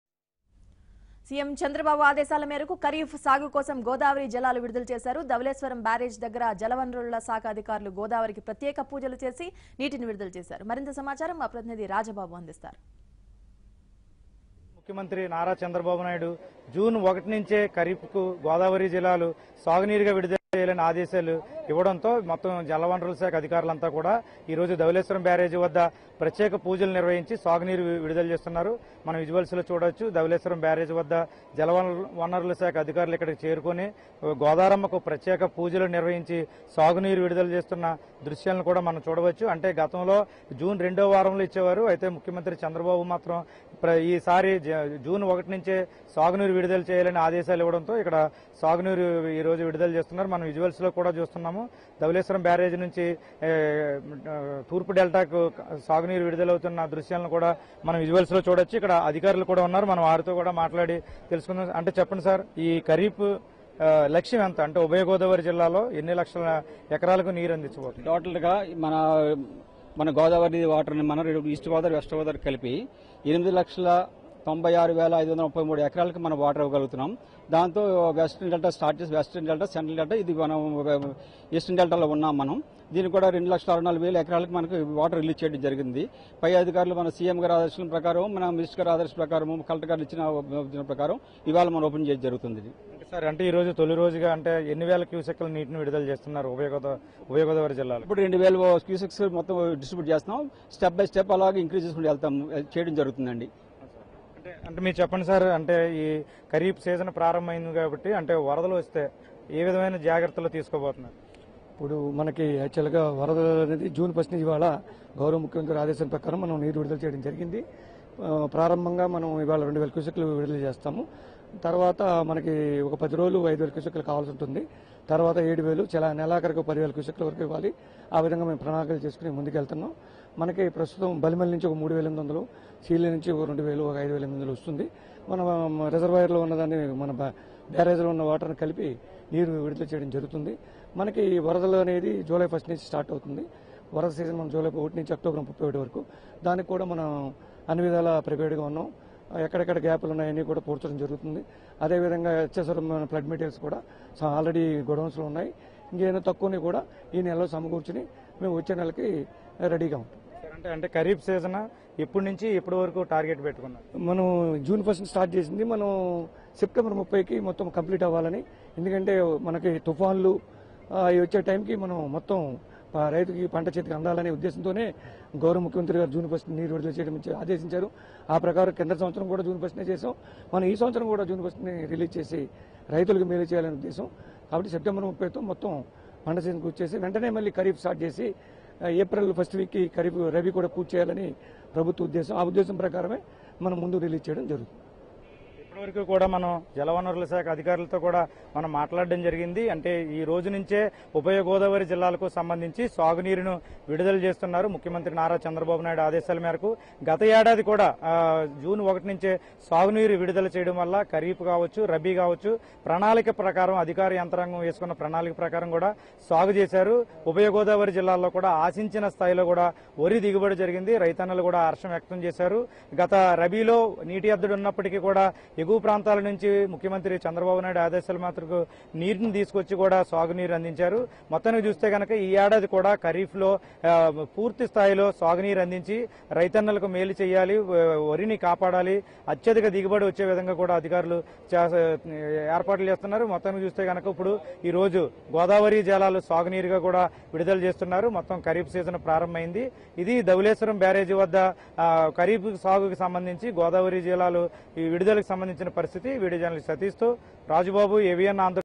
Officials Release Godavari Water For Kharif Crops In Dowleswaram | ABN Telugu
Officials release Godavari water for Kharif crops in Dowleswaram  to benefit the farmers and their fields by the government.